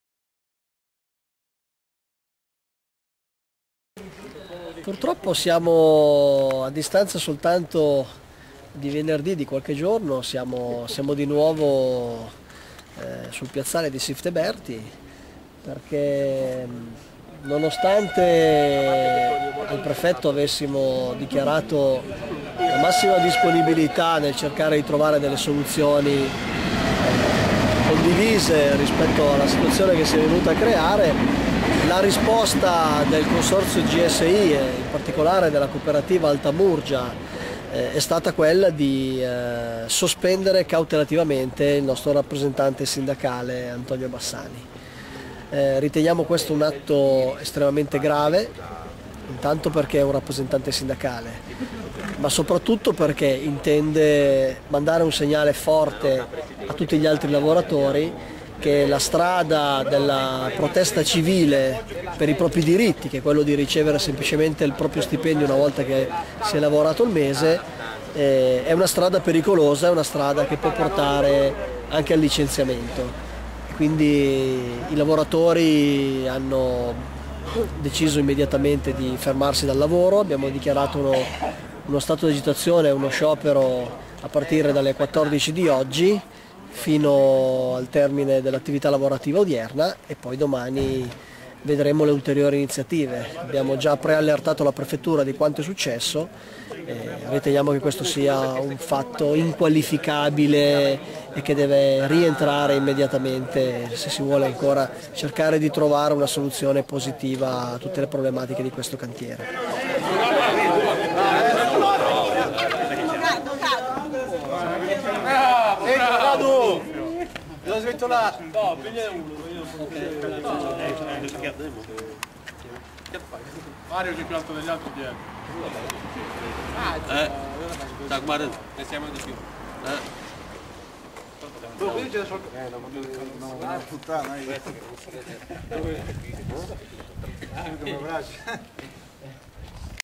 CONTINUA LO SCIOPERO IN SIFTE BERTI - Intervista a Luca Trinchitella, di Cisl Fisascat
Ha parlato troppo. Ha detto cose "lesive" nei confronti dell'azienda e così, via fax, è stata inviata una lettera di sospensione ad Antonio Bassani, Rsa Fisascat Cisl. Venerdì era in protesta insieme ad altri colleghi davanti alla Prefettura ed ha esposto le motivazioni del loro sciopero di otto ore. Questo non è stato visto di buon grado dal Consorzio Gruppo Servizi Italia (con sede in Via Trino a Vercelli) in particolare della Cooperativa Altamurgia, ed è stato ufficialmente sospeso in via cautelativa.  Sul piazzale di Sifte Berti, i dipendenti hanno manifestato il loro disagio, esprimendo solidarietà a Bassani. "Riteniamo questo un atto estremamente grave -- rimarca Luca Trinchitella, di Cisl Fisascat- intanto perché è un rappresentante sindacale, ma soprattutto perché questo gesto intende mandare un segnale forte agli altri dipendenti"